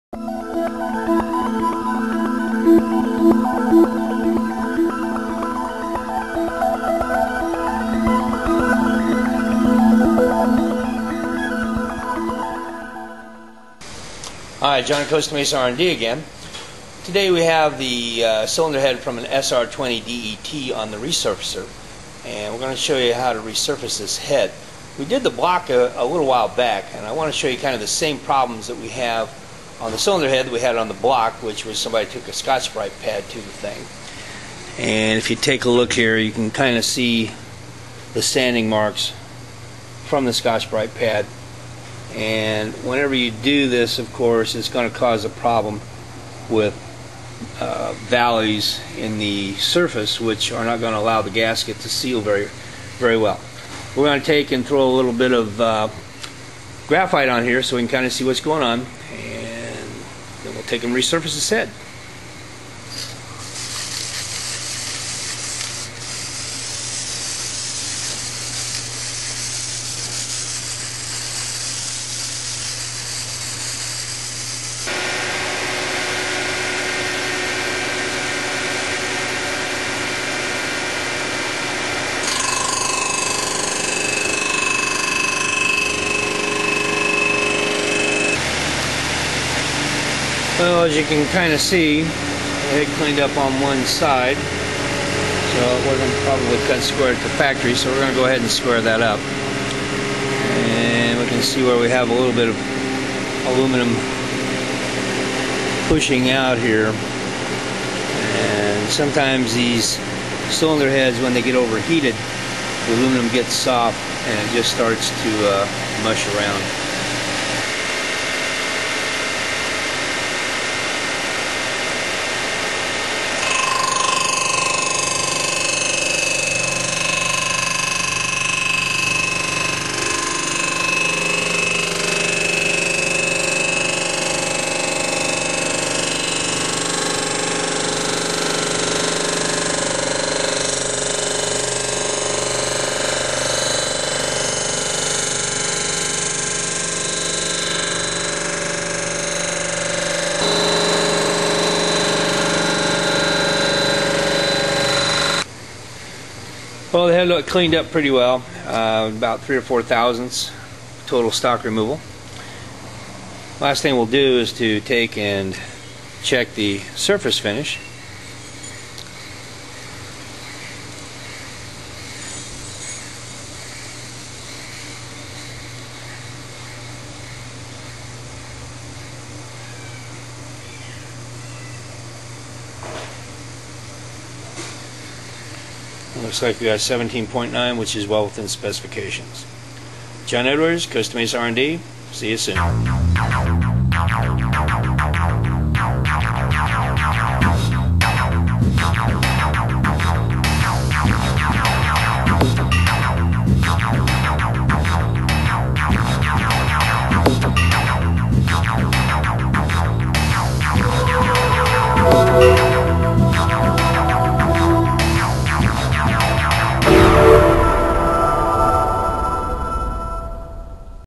Nissan SR-20 DET Head Resurface
John Edwards describes how he resurfaces the popular Nissan SR-20 DET cylinder head from Costa Mesa R&D Automotive Machine.